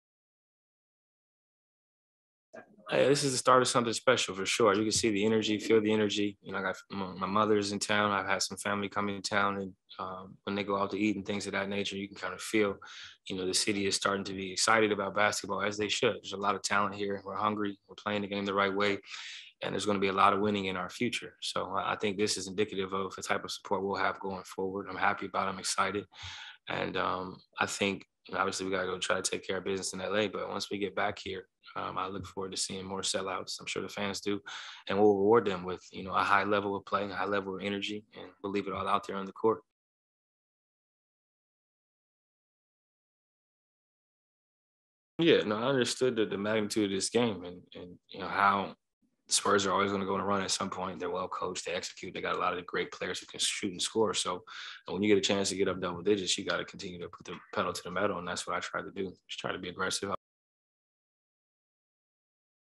"This is a start of something special" McCollum says after Pelicans' play-in win
CJ McCollum got New Orleans going early and Brandon Ingram took over late to send the Pelicans to Los Angeles for a chance at the final playoff spot.

McCollum scored 27 of his 32 points in the first half to carry the young Pelicans to a 113-103 win over the San Antonio Spurs on Wednesday night. New Orleans will face the LA Clippers on Friday night for the eighth playoff seed.

Playing in his 57th playoff game, McCollum displayed a calming presence to his younger teammates. Rookies Herb Jones, Jose Alvarado and Trey Murphy played large chunks of the fourth quarter.

"I understood the magnitude of this game," McCollum said. "The Spurs are always going to go on a run at some point. They are well-coached. They execute. They have a lot of great players who can shoot and score. When you get a chance to get up double digits, you have to put the pedal to the metal and try and be aggressive."